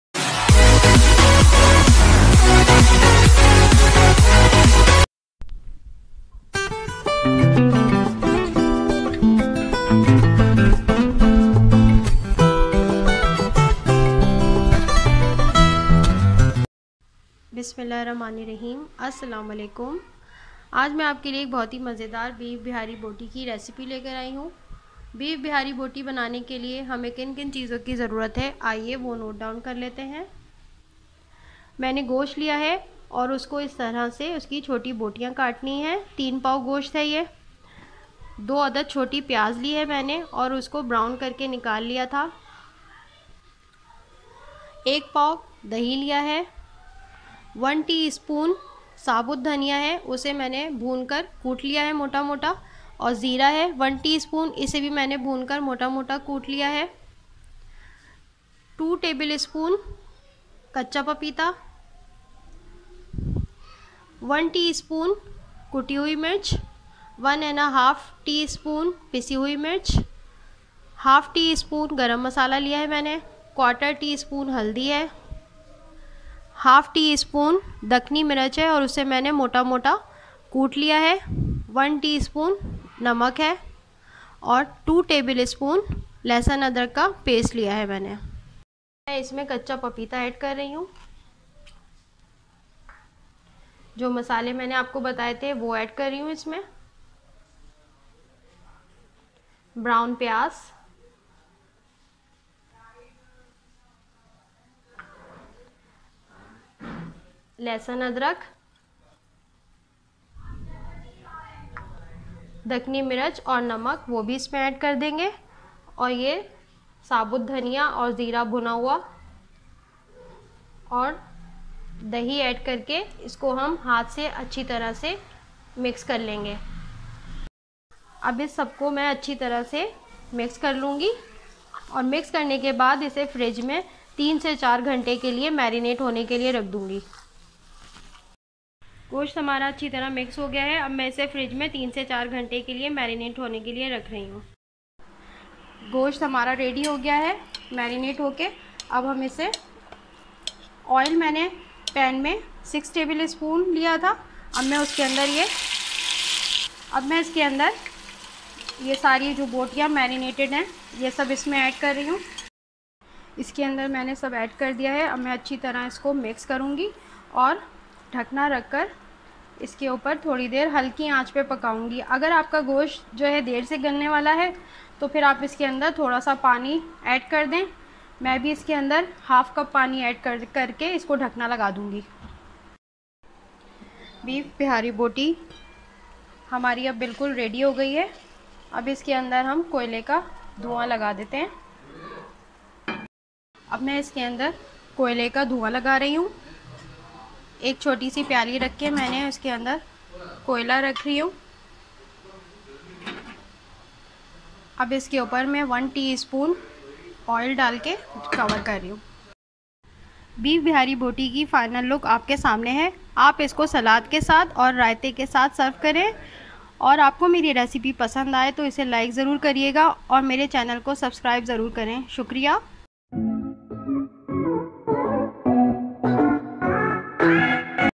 Beef bihari boti recipe |how to make beef bihari boti|f for foodie
Another simple and easy recipe of beef bihari boti. Try this and enjoy ❤️